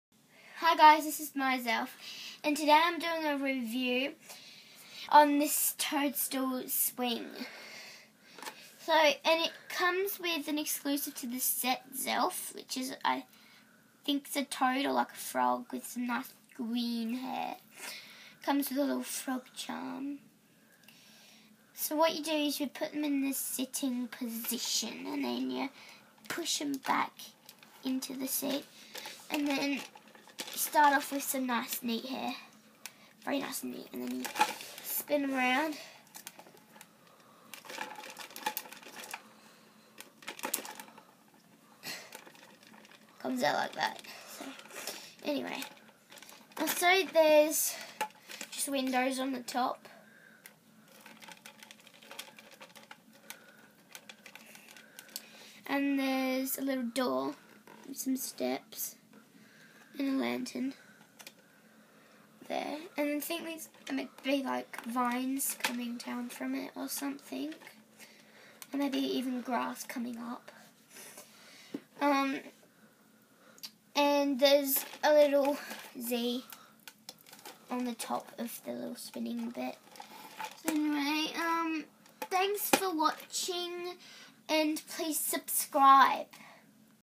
Zelf Toadstool Twirl and Swing Playset - Review
Hey guys! Today i'm reviewing the zelf toadstool playset.
Which comes with 1 special edition zelf, and the toadstool swing.